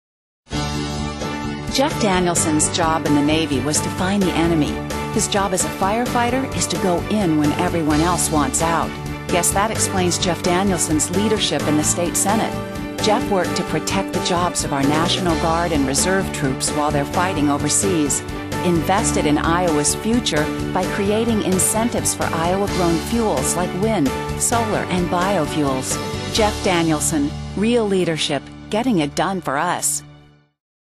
Jeff Danielson: Leadership in the Iowa Senate
Jeff Danielson:
A Navy veteran, a firefighter and an Iowa State Senator;
protecting our soldiers jobs while overseas and
creating incentives for Iowa grown fuels.
Real leadership for Iowa.